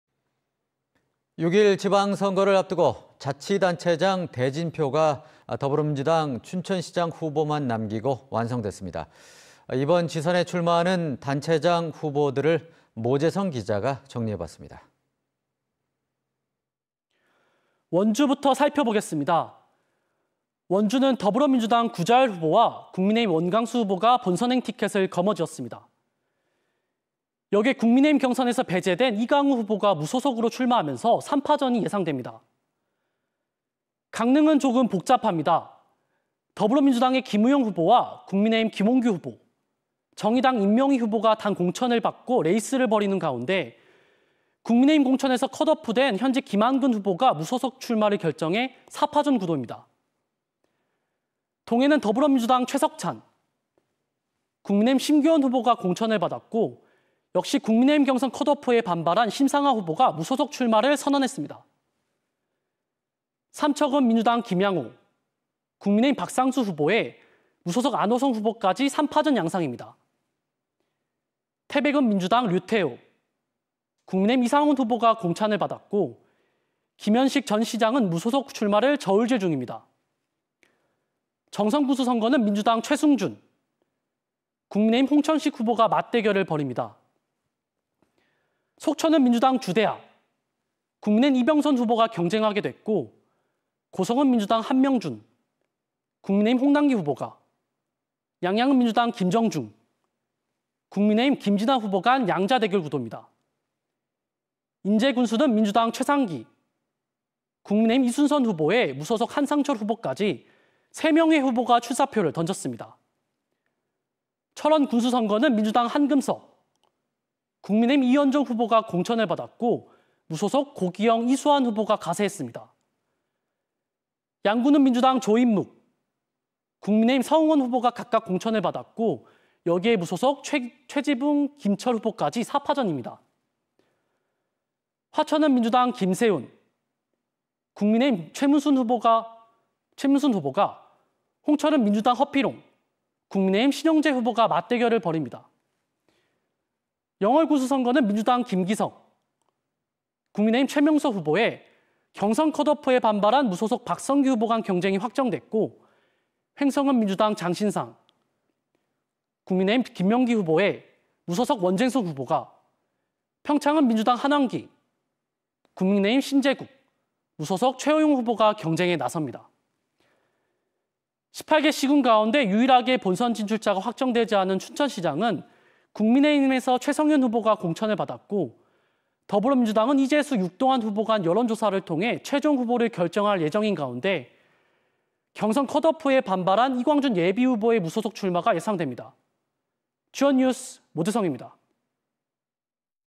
[G1뉴스] 지방선거 D-30, 기초단체장 대진표 확정
[앵커]
6·1 지방선거를 앞두고 자치단체장 대진표가, 더불어민주당 춘천시장 후보만 남기고 완성됐습니다.

이번 지선에 출마하는 단체장 후보들을 모재성 기자가 정리했습니다.

[리포터]
원주부터 살펴보겠습니다.

/원주는 더불어민주당 구자열 후보와 국민의힘 원강수 후보가 본선행 티켓을 거머쥐었습니다.

여기에 국민의힘 경선에서 배제된 이강후 후보가 무소속으로 출마하면서 3파전이 예상됩니다./

/강릉은 조금 복잡합니다.

더불어민주당의 김우영 후보와 국민의힘 김홍규 후보, 정의당 임명희 후보가 당 공천을 받고 레이스를 벌이는 가운데,

국민의힘 공천에서 컷오프된 현직 김한근 후보가 무소속 출마를 결정해 4파전 구도입니다./

/동해는 더불어민주당 최석찬, 국민의힘 심규언 후보가 공천을 받았고, 역시 국민의힘 경선 컷오프에 반발한 심상화 후보가 무소속 출마를 선언했습니다.

삼척은 민주당 김양호, 국민의힘 박상수 후보에 무소속 안호성 후보까지 3파전 양상입니다.

태백은 민주당 류태호, 국민의힘 이상호 후보가 공천을 받았고, 김연식 전 시장은 무소속 출마를 저울질 중입니다.

정선군수 선거는 민주당 최승준, 국민의힘 홍천식 후보가 맞대결을 벌입니다./

/속초는 민주당 주대하, 국민의힘 이병선 후보가 경쟁하게 됐고,

고성은 민주당 함명준, 국민의힘 홍남기 후보가, 양양은 민주당 김정중, 국민의힘 김진하 후보간 양자 대결 구도입니다.

인제군수는 민주당 최상기, 국민의힘 이순선 후보에 무소속 한상철 후보까지 3명의 후보가 출사표를 던졌습니다./

/철원군수 선거는 민주당 한금석, 국민의힘 이현종 후보가 공천을 받았고, 무소속 고기영, 이수환 후보가 가세했습니다.

양구는 민주당 조인묵, 국민의힘 서흥원 후보가 각각 공천을 받았고, 여기에 무소속 최지붕, 김 철 후보까지 4파전 입니다.

화천은 민주당 김세훈, 국민의힘 최문순 후보가, 홍천은 민주당 허필홍, 국민의힘 신영재 후보가 맞대결을 벌입니다./

/영월군수 선거는 민주당 김기석, 국민의힘 최명서 후보에, 경선 컷오프에 반발한 무소속 박선규 후보간 경쟁이 확정됐고,

횡성은 민주당 장신상, 국민의힘 김명기 후보에 무소속 원재성 후보가, 평창은 민주당 한왕기, 국민의힘 심재국, 무소속 최호영 후보가 경쟁에 나섭니다./

/18개 시·군 가운데 유일하게 본선 진출자가 확정되지 않은 춘천시장은 국민의힘에서 최성현 후보가 공천을 받았고,

더불어민주당은 이재수, 육동한 후보간 여론조사를 통해 최종 후보를 결정할 예정인 가운데, 경선 컷오프에 반발한 이광준 예비후보의 무소속 출마가 예상됩니다./

G1뉴스 모재성입니다.